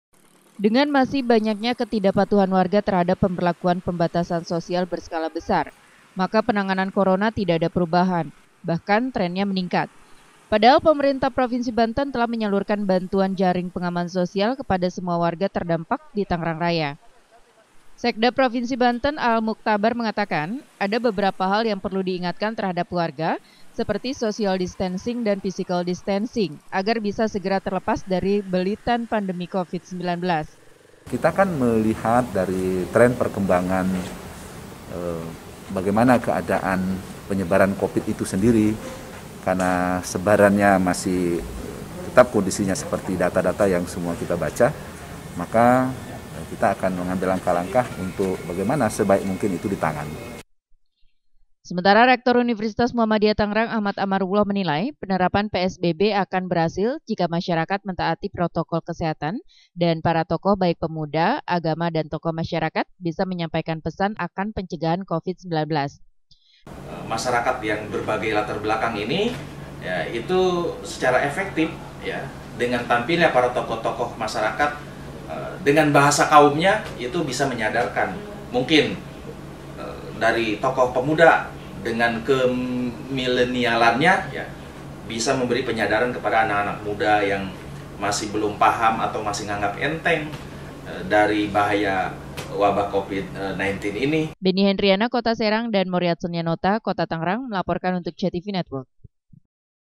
PSBB Tangerang Raya Belum Efektif Tekan Covid-19
Kota Tangerang, PEMBATASAN Sosial Berskala Besar di Tangerang Raya dinilai belum efektif tekan Covid-19, Pemerintah Provinsi Banten berkali-kali mengingatkan warga terdampak mematuhi protokol kesehatan, agar penyebaran Corona dapat dihentikan.